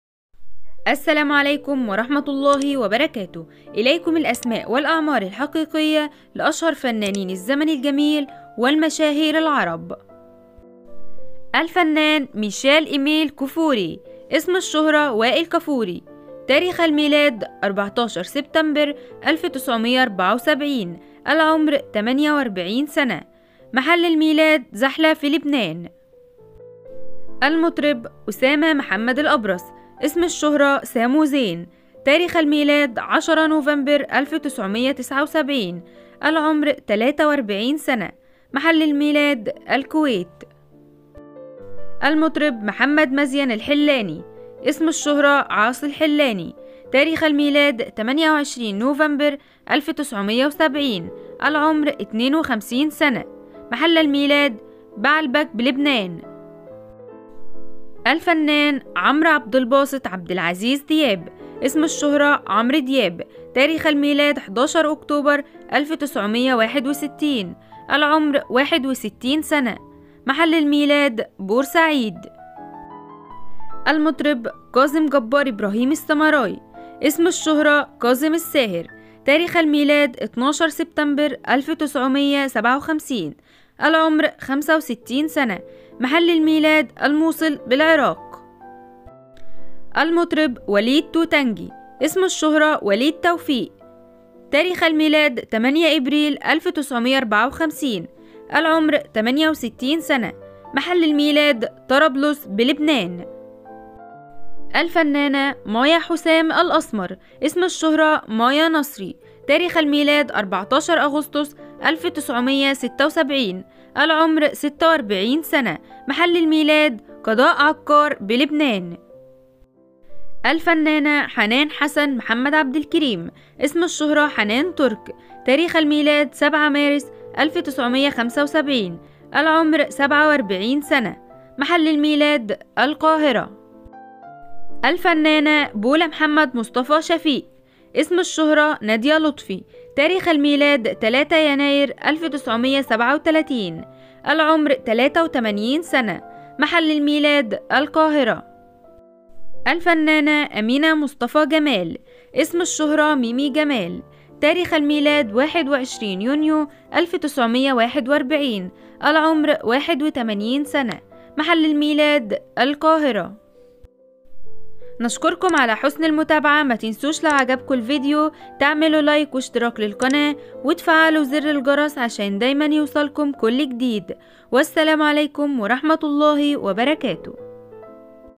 مش هتصدق الاسماء والاعمار الحقيقية للفنانين والمشاهير العرب
الاسماء والاعمار الحقيقية للفنانين والمشاهير العرب
الفنان عمرو دياب
الفنان وائل كفوري
الفنان كاظم الساهر
الفنان ساموزين
عاصي الحلاني
وليد توفيق
مايا نصري
حنان ترك
نادية لطفي
ميمي جمال